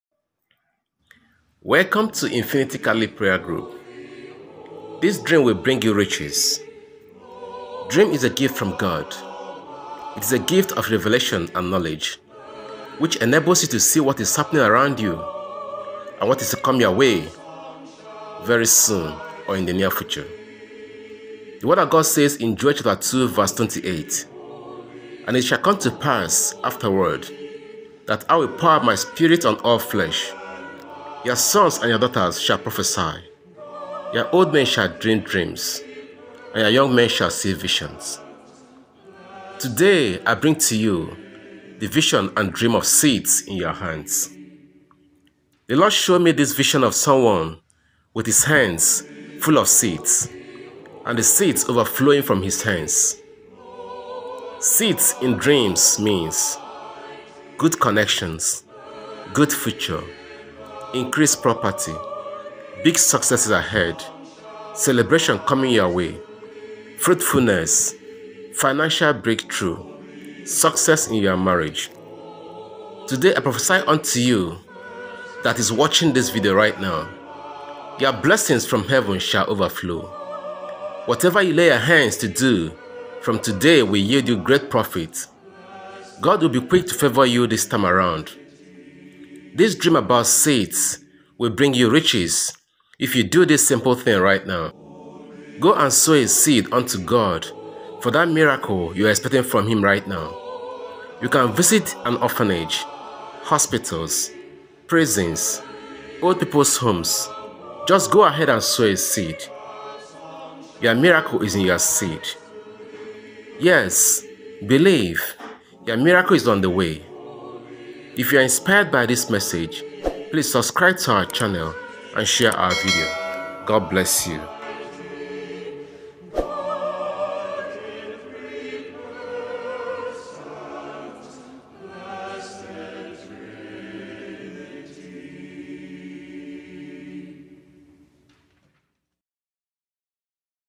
This Dream Will Bring You Riches; If You Do This Right Now
propheticmessage propheticword dreams riches THIS DREAM WILL BRING YOU RICHES. Dream is a gift from God. It is a gift of revelation and knowledge; which enables you to see what is happening around you and what is to come your way soon or in near future.

The word of God says in Joel 2:28 “And it shall come to pass afterward, that I will pour out my Spirit on all flesh; your sons and your daughters shall prophesy, your old men shall dream dreams, and your young men shall see visions.

Today I bring to you the vision and dream of SEEDS in your hands.

THIS DREAM WILL BRING YOU RICHES